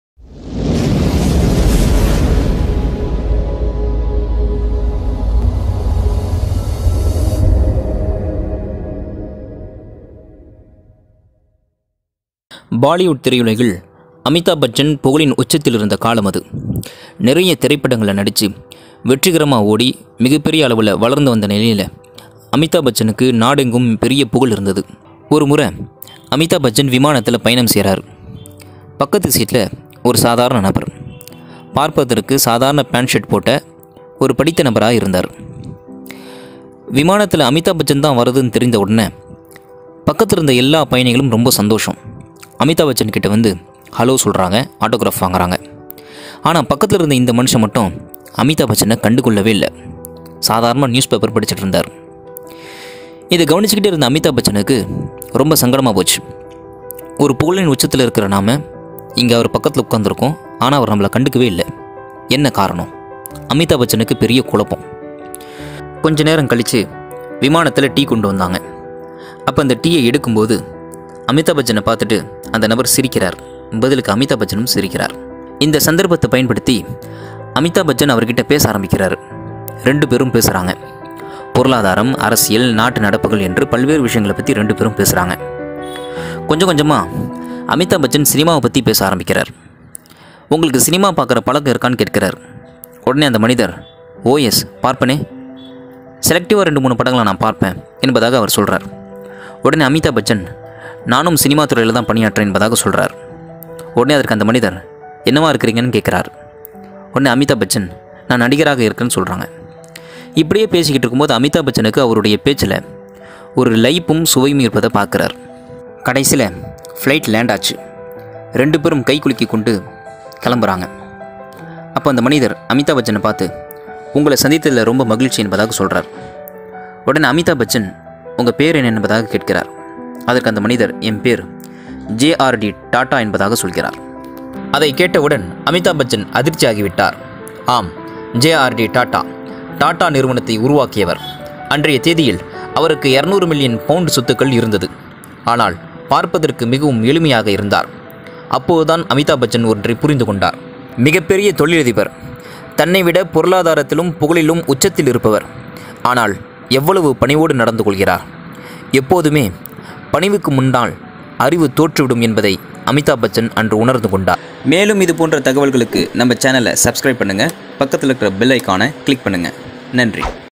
Amitab Bachan got Shocked | அதிர்ச்சியடைந்த அமிதாப் பச்சன், காரண்ம் என்ன? | Master Voice
Amitab met JRD TATA in flight. Inspirational Incident